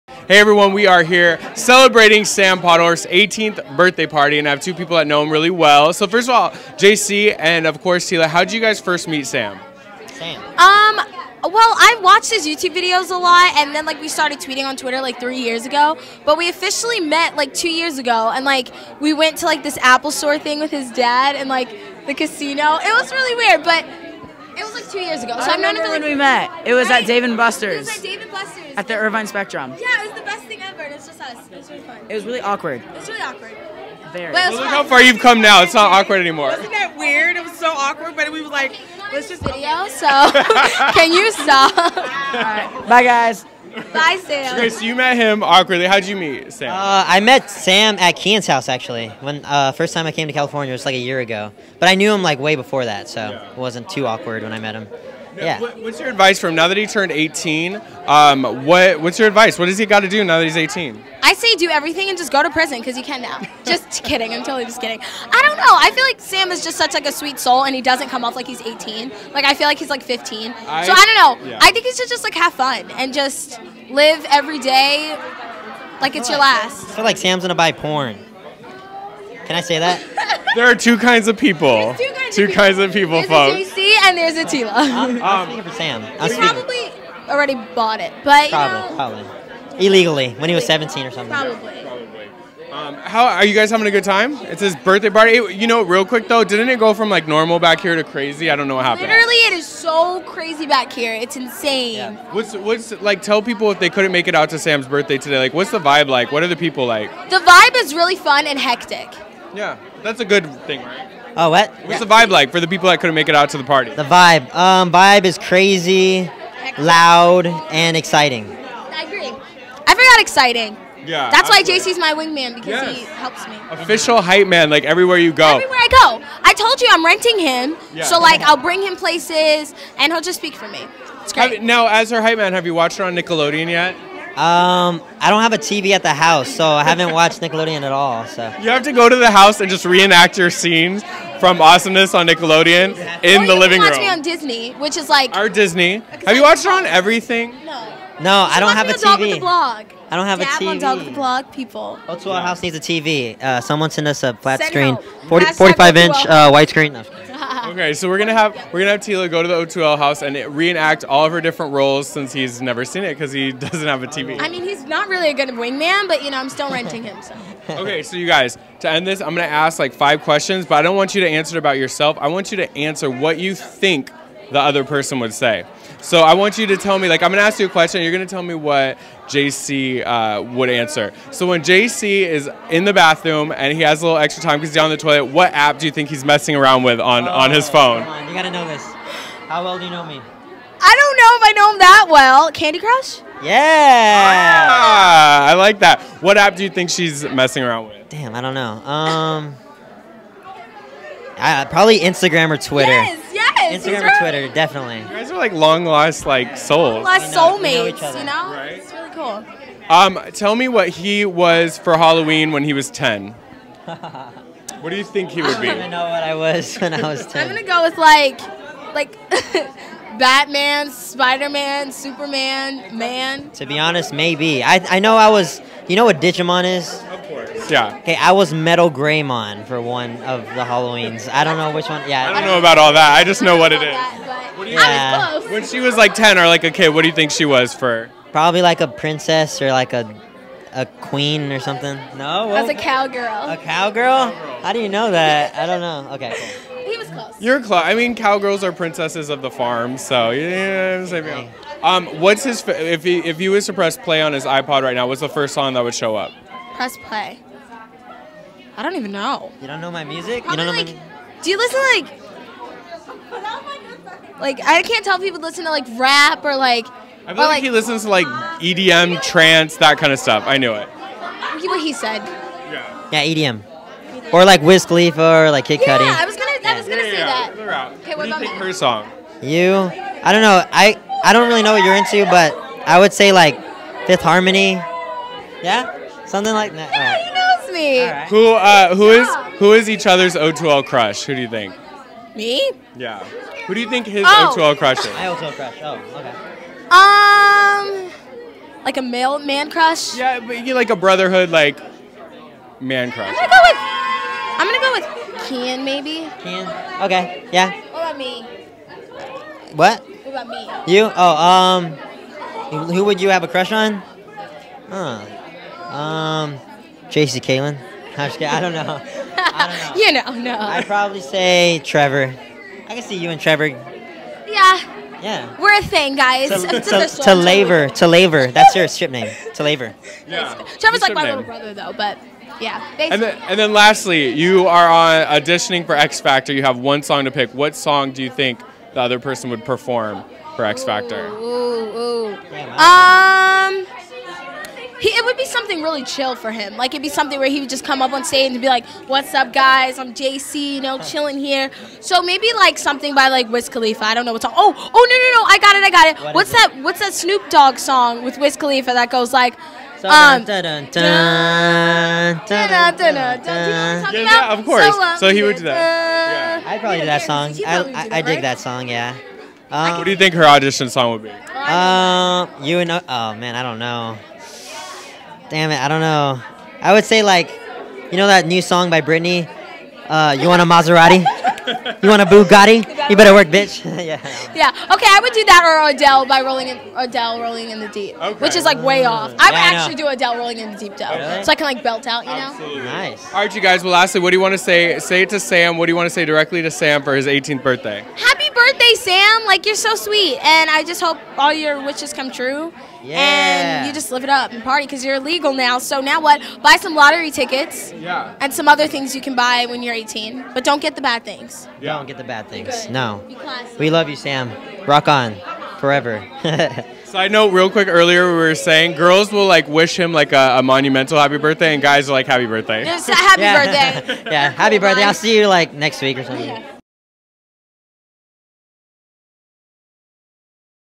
JC Caylen and Teala Dunn at Sam Pottorff 18Th Bday
Pacific Rim Video correspondent Chris Trondsen interviews JC Caylen of O2L and Teala Dunn during Sam Pottorff's 18th Birthday Party held at The Avalon in Hollywood, CA on Sunday, October 27, 2013.